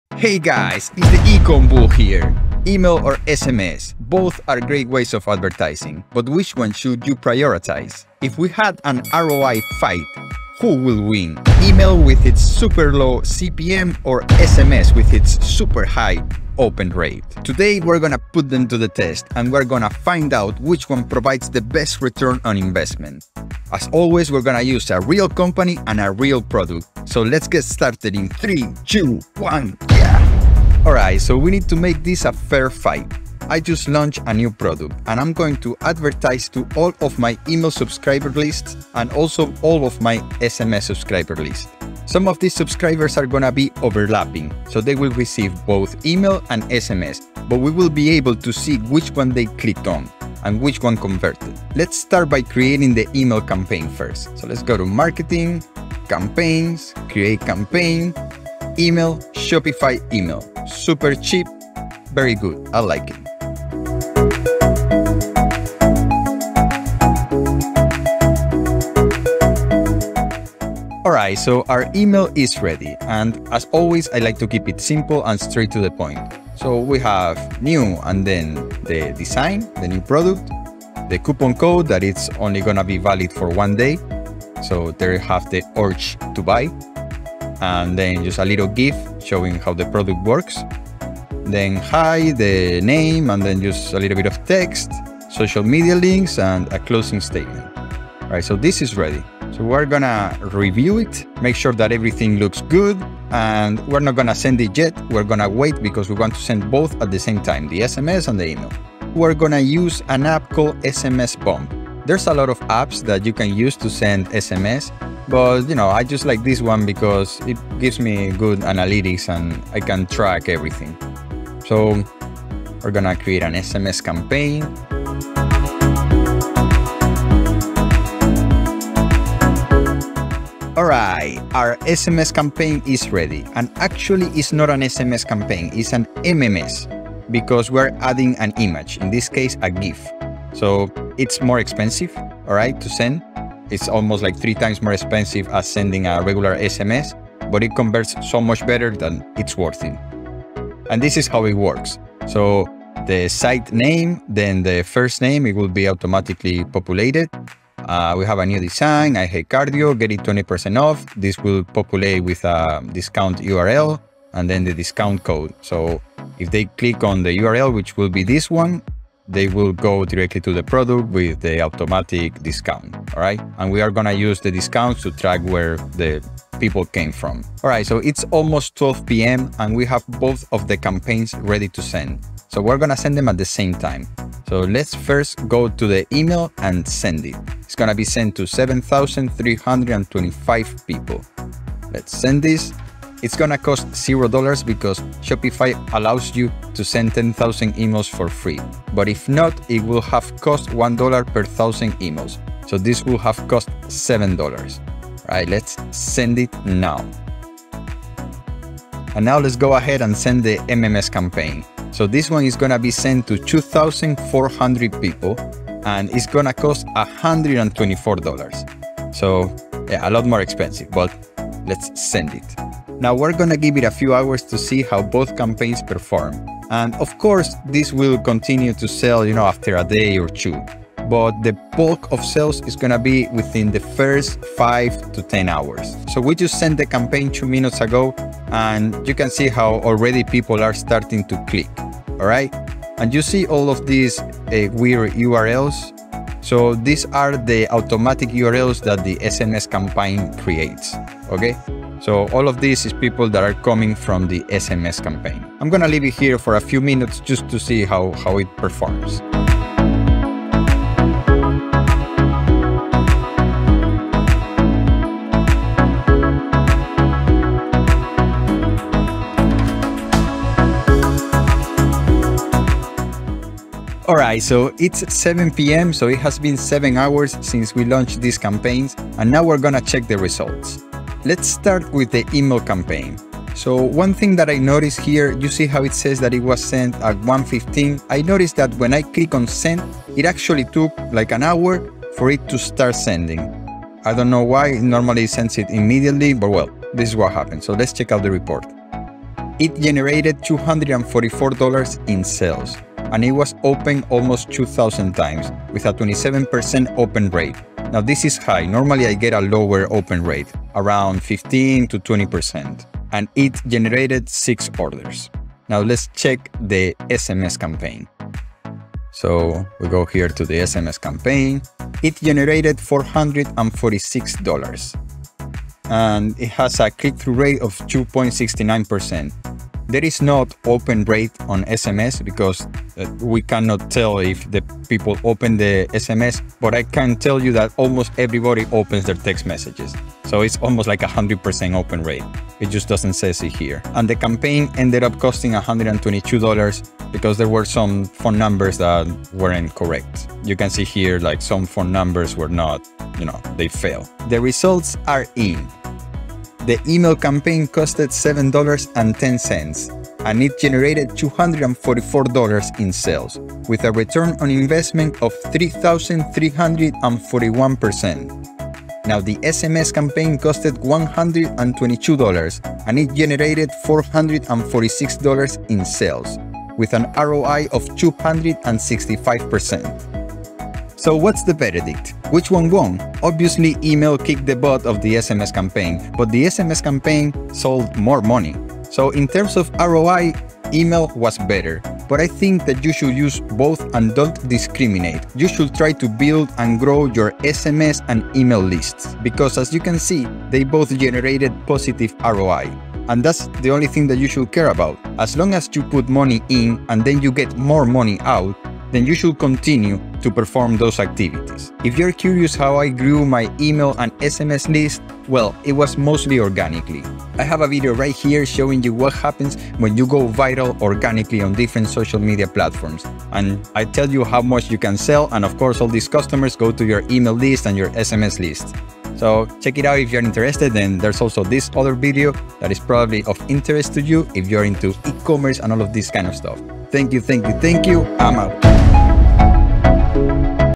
How to SELL using SMS Marketing on Shopify
In this video, we will compare Email Marketing to SMS Marketing.
We will test an email campaign and an SMS campaign side by side to determine which provides the best ROI.

Information on:
email marketing strategy, how to build an email list, klaviyo alternative, Shopify email marketing

Chapters:
00:00 Email VS SMS intro
00:30 Creating the Email Campaign
01:49 Creating the SMS Campaign
02:57 Sending the Campaigns
04:33 Reviewing the Reports
05:54: Results
6:20: Verdict

If you have any questions leave them in the comments and I will try to help you.